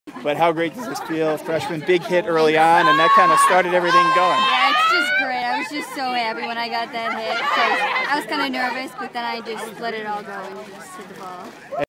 Nicki Fitzgerald on Mount Mercy's win
Live from the Monsignor Martin softball championships with The Buffalo News Prep Talk blog. For more go to buffalonews.com.